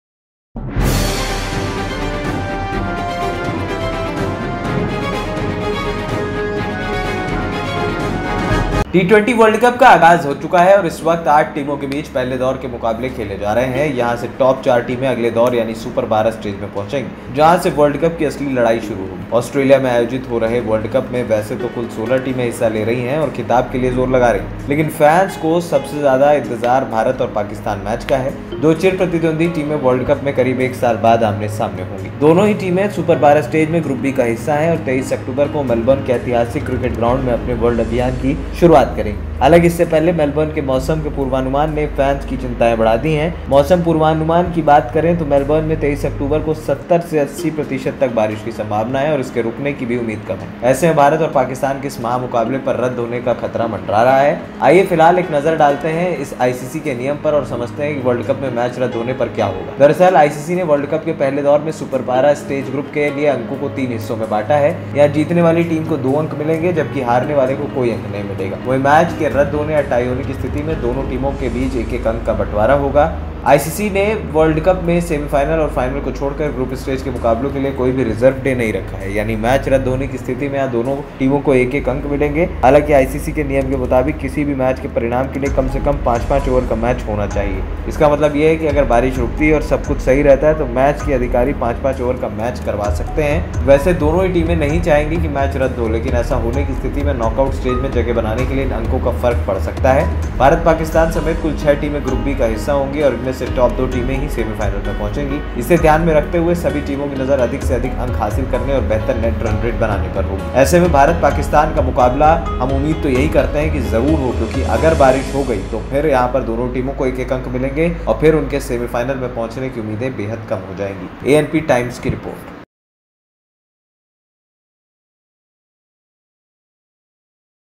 BIGBREAKING:भारत-पाक मुकाबला होगा रद्द,करोड़ों फैंस को लगा तगड़ा झटका,ICC को हुआ भारी नुक्सान
इस ख़ास रिपोर्ट में देखें भारत-पाक मुकाबला होगा रद्द,करोड़ों फैंस को लगा तगड़ा झटका,ICC को हुआ भारी नुक्सान

ANPTIMES
anptimes